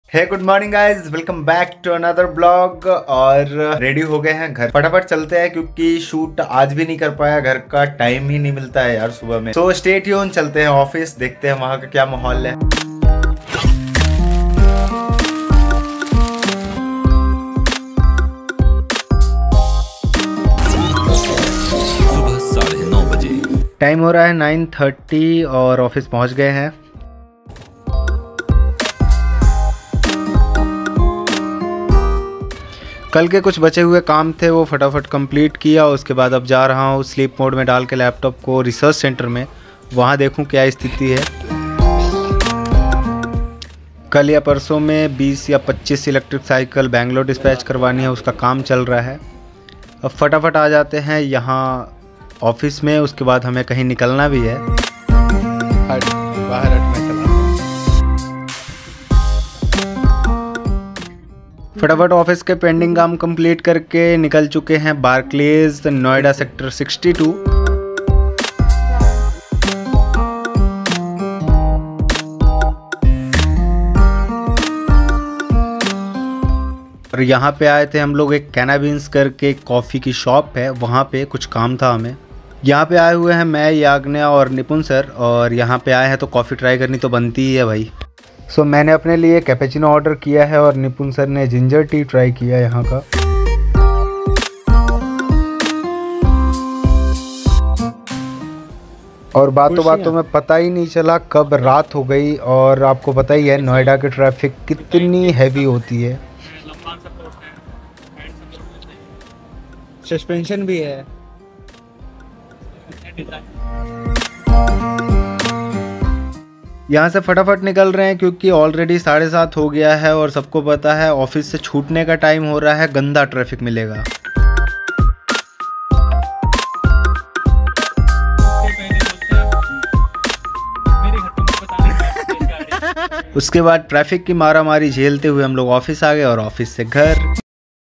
"Noida Barclays Office: A Day of Productivity and Coffee"
For YouTube, you can use the following description:

"Welcome to my virtual tour of the Noida Barclays office, where I share my day filled with work, camaraderie, and the delightful aroma of coffee. Come along as we explore the world of finance and enjoy a cup of coffee with us in this exciting video adventure.